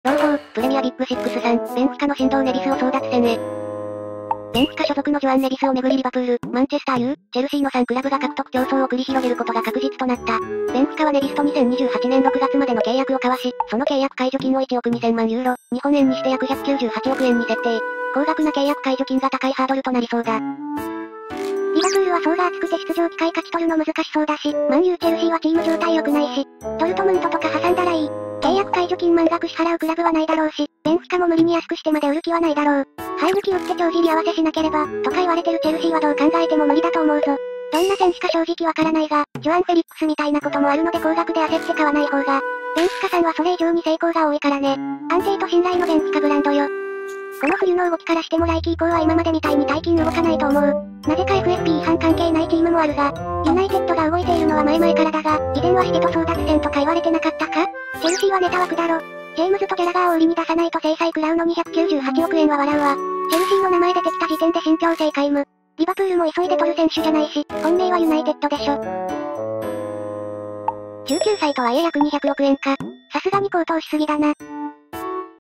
【朗報】プレミアBIG6さん、ベンフィカの神童ネビスを198億円で争奪戦へwwwwwwwwwwwww
▼ソース